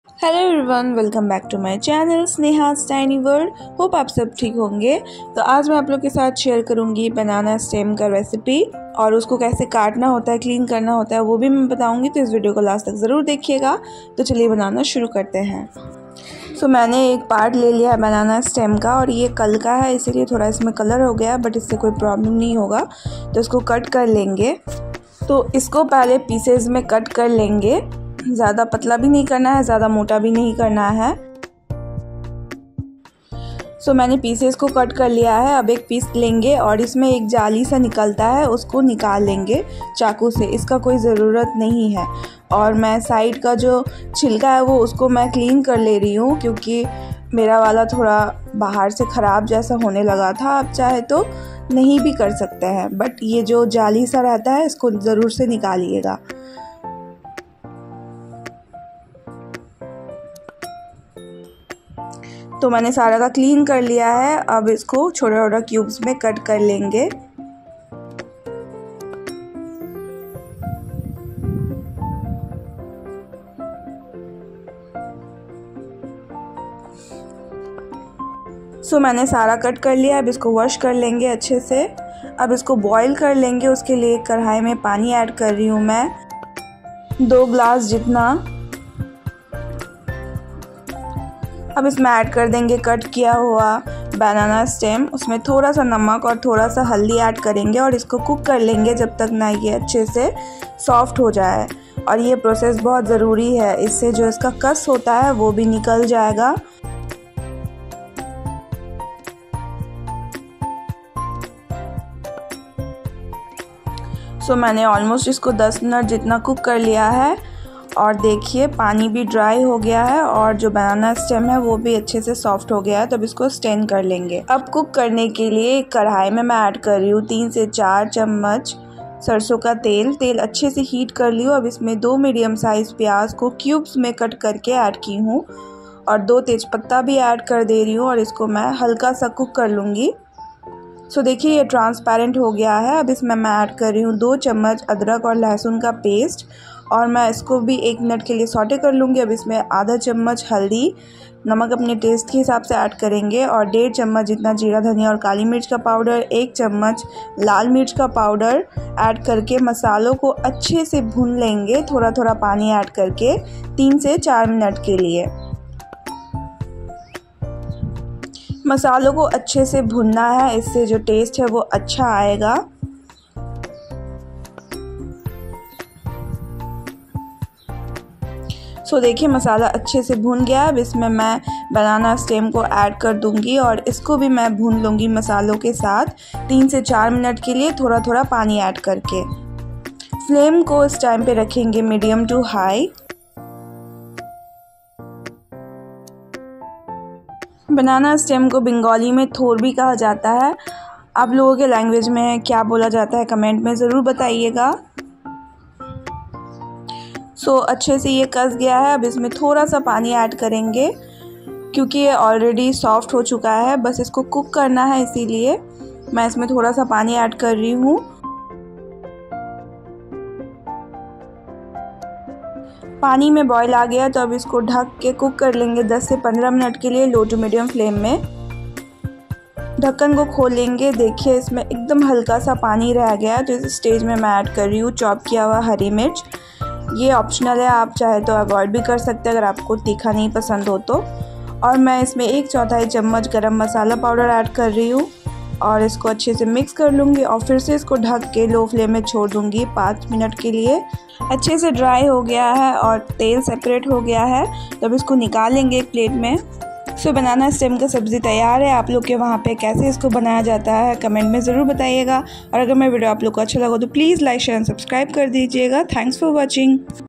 Banana Stem Recipe....Cooking & cutting step by step...Best for Lunch👍
Grandmother's recipe...This  recipe is very easy and healthy...Must try recipe...

Instagram I'd - snehastinyworld
Facebook page - sneha's tiny world